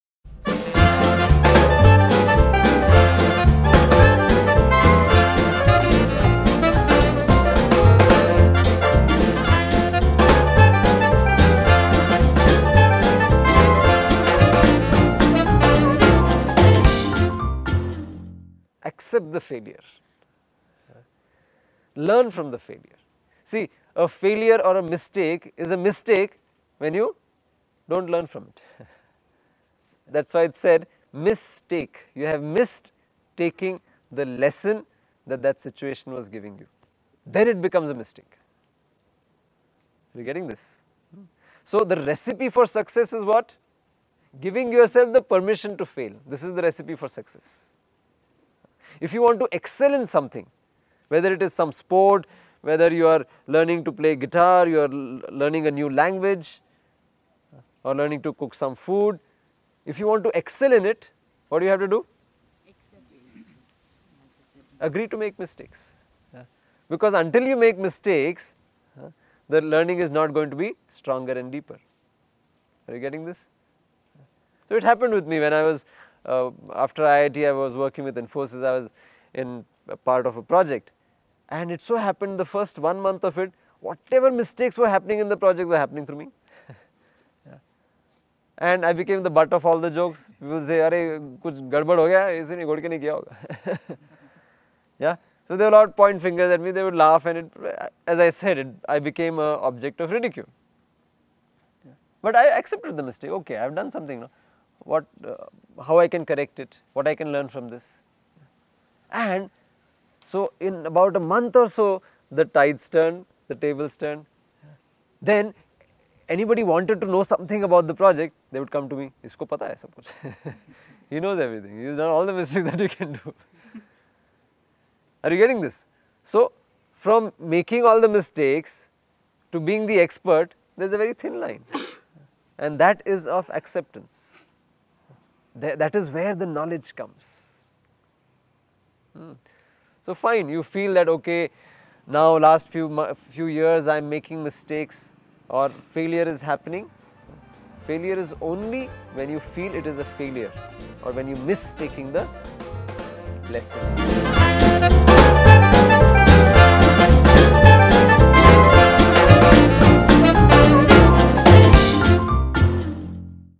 Mistakes can bring you Success : Ask Bawa and Dinesh 7
This video is for the ones who want to know how excel in anything and become successful.

Khurshed Batliwala and Dinesh Ghodke,  Faculties of Art of Living and Authors of Ready, Study, Go! talks on how to deal with the most common problems that most of us face which can be  effortlessly integrated into daily life.